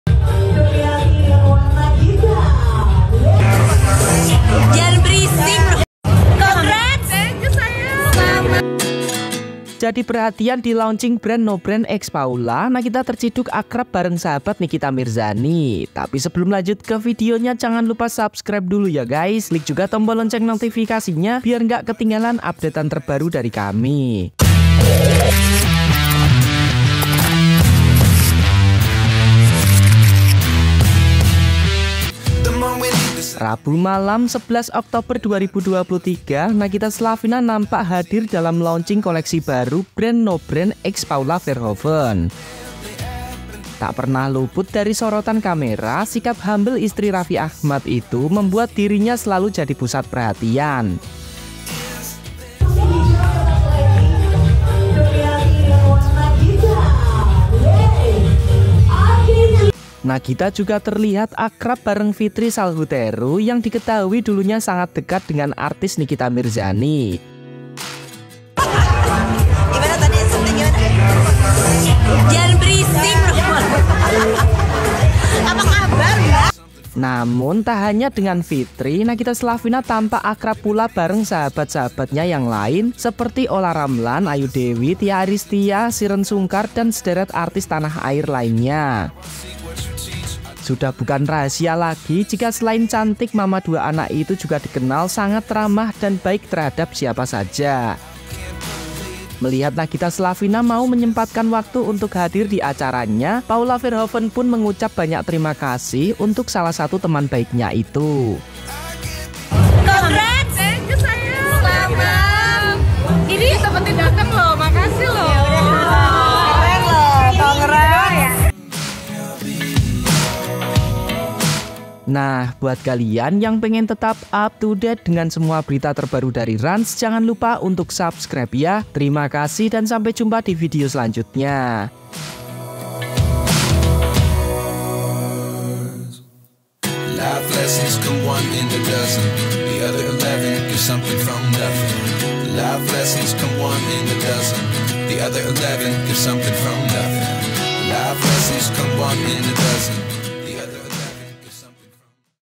JadiPERHATIAN diLAUNCHING BRAND NO BRAND X PAULA,Nagita TERCIDUK AKRAB BARENG SAHABAT NIKITA MIRZANI
Jadi PERHATIAN diLAUNCHING BRAND NO BRAND X PAULA, Nagita TERCIDUK AKRAB BARENG SAHABAT NIKITA MIRZANI

Rabu malam 11 Oktober 2023, Nagita Slavina nampak hadir dalam launching koleksi terbaru Brand No Brand X Paula Verhouven.

Tak pernah luput dari sorotan kamera, sikap humble istri Raffi Ahmad itu membuat dirinya selalu jadi pusat perhatian..

Nagita, juga terlihat akrab bareng Fitri Salhuteru yang diketahui dulunya sangat dekat dengan artis Nikita Mirzani.

Namun, Tak hanya dengan Fitri, Nagita slavina tampak akrab pula bareng sahabat sahabatnya yang lain, seperti Olla Ramlan, Ayu Dewi, Tya Ariestya, Shireen Sungkar, dan sederet artis tanah air lainnya..

Sudah bukan rahasia lagi, jika selain cantik mama 2 anak itu juga dikenal sangat ramah dan baik terhadap siapa saja..

Melihat Nagita slavina mau menyempatkan waktu untuk hadir di acaranya, Paula Verhouven pun mengucap banyak terimakasih untuk salah satu teman baiknya itu..